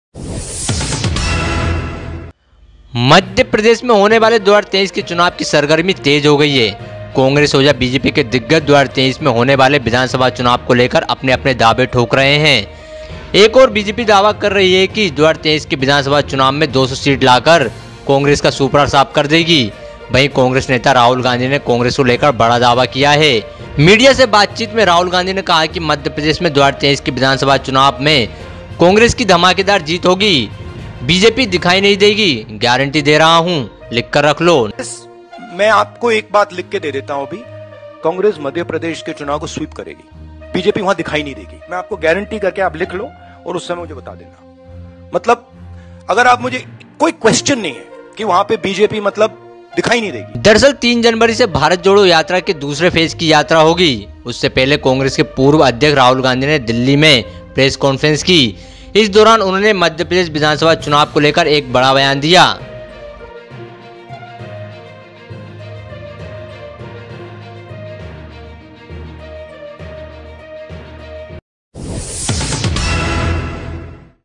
Rahul Gandhi ने खोले Congress के जीत के पत्ते
मध्य प्रदेश में होने वाले 2023 के चुनाव की सरगरमी तेज हो गई है कांग्रेस और बीजेपी के दिग्गज 2023 में होने वाले चुनाव को लेकर अपने अपने दावे ठोक रहैं है एक बीजेपी दावा कर रही है